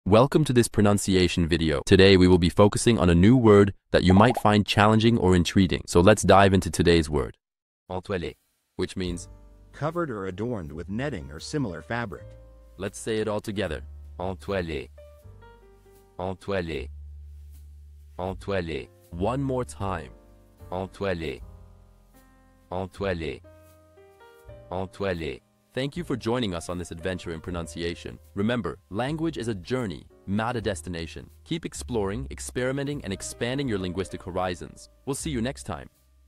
How to pronounce Entoilé
Master the Pronunciation of 'Entoilé Which means spin' - which means : Covered or Adorned with netting or similar manufacturing. 📝 with @BaguettePronunciation 🎓🗣️ - [Your Guide to French]

Learn to pronounce 'Entoilé' perfectly in French with our easy and engaging tutorial by @BaguettePronunciation! 📖📽️ In this comprehensive video, we provide you with native speaker examples 🗣️👂 and phonetic breakdowns 🎵 to ensure you get the pronunciation just right.

How to say Spin in French